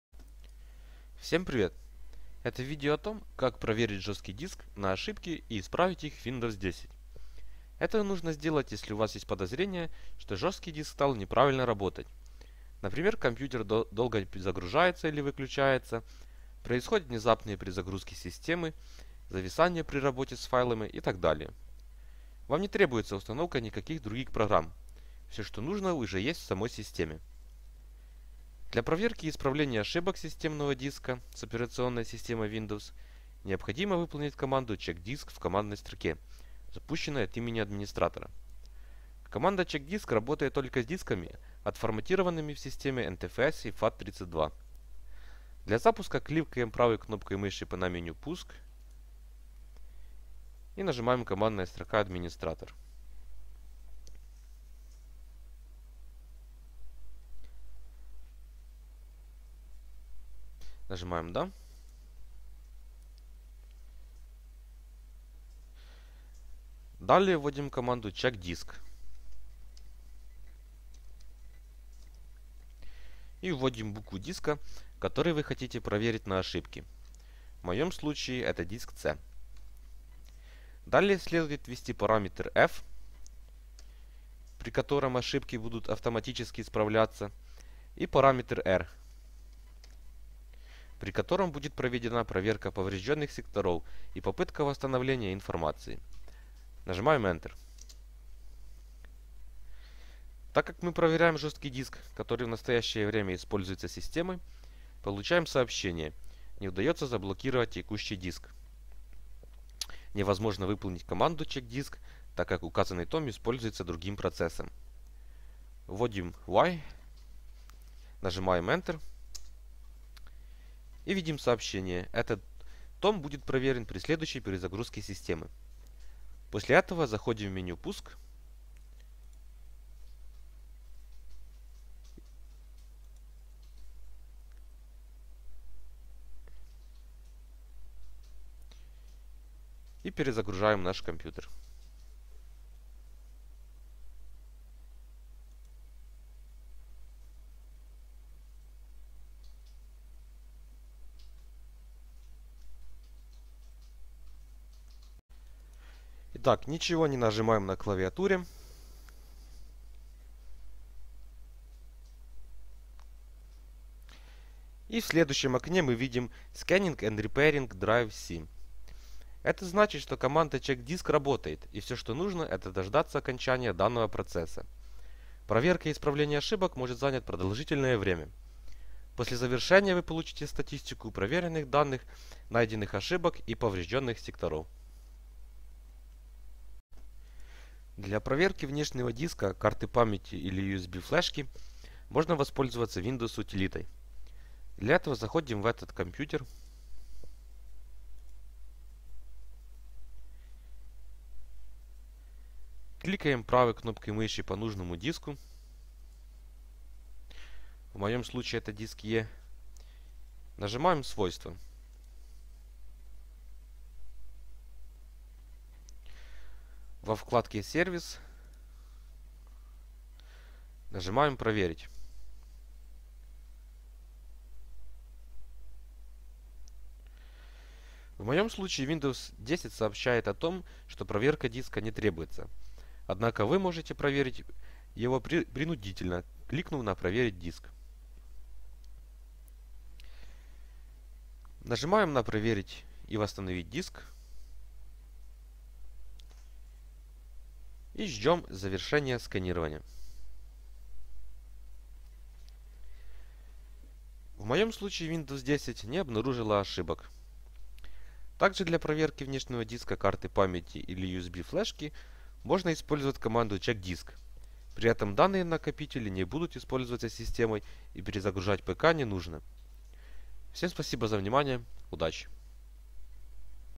Как проверить жесткий диск на ошибки и исправить ошибки в Windows 10, 8, 7 🔎🛠️🗄️
👍 Смотрите как проверить жесткий диск на ошибки и исправить их в Windows 10. Это нужно сделать, если у вас есть подозрения, что жесткий диск стал неправильно работать.

Читайте: Как восстановить жесткий диск с помощью CHKDSK в Windows 10

На пример компьютер долго загружается или выключается, происходят внезапные перезагрузки системы, зависания при работе с файлами и т.д. Вам не требуется установка никаких других программ, все что нужно уже есть в самой системе.

Содержание:
00:00 - Вступление;
0:28 - Проверка и исправления ошибок системного диска
3:07 - Проверки внешнего диска, карты памяти или USB флешки

Для проверки и исправления ошибок системного диска (с операционной системой Windows) необходимо выполнить команду chkdsk в командной строке, запущенной от имени администратора. Команда chkdsk работает только с дисками, отформатированными в системе NTFS или FAT32.

Для запуска кликаем правой кнопкой мыши на меню "Пуск" и нажимаем "Командная строка (администратор)". Далее вводим команду "chkdsk" и вводим букву диска, который вы хотите проверить на ошибки (в моем случае это диск С). Далее следует ввести параметр "/f", при котором ошибки будут автоматически исправляться и параметр "/r", при котором будет проведена проверка поврежденных секторов и попытка восстановления информации. Нажимаем "Enter".

Так как мы проверяем жесткий диск, который в настоящее время используется системой, получаем сообщение: "Не удается заблокировать текущий диск. Невозможно выполнить команду chkdsk так как указанный том используется другим процессом".

Вводим "Y", нажимаем "Enter" и видим сообщение: "Этот том будет проверен при следующей перезагрузке системы". После этого, заходим в меню "Пуск" и перезагружаем наш ПК.
Ничего не нажимаем на клавиатуре и в следующем окне мы видим: "Scanning and repairing drive (C:) ". Это значит, что команда "chkdsk" работает, и все что нужно – это дождаться окончания данного процесса. Проверка и исправление ошибок может занять продолжительное время. После завершения вы получите статистику проверенных данных, найденных ошибок и поврежденных секторов.

Для проверки внешнего диска, карты памяти или USB флешки можно воспользоваться Windows утилитой. Для этого, заходим в "Этот компьютер", кликаем правой кнопкой мыши по нужному диску (в моем случае это диск E), выбираем пункт "Свойства". Во вкладке "Сервис" нажимаем "Проверить". В моем случае Windows 10 сообщает о том, что проверка диска не требуется, однако вы можете проверить его принудительно, кликнув на "Проверить диск". Нажимаем на "Проверить диск" и ждем завершения сканирования. В моем случае Windows не обнаружила ошибок. Также для проверки внешнего диска, карты памяти или USB флешки можно использовать команду chkdsk. При этом данные накопители не будут использоваться системой и перезагружать ПК не нужно. Всем спасибо за внимание, удачи.

Смотрите другие ролики на тему: Windows, chkdsk, HDD, NTFS, FAT, БитыйСектор.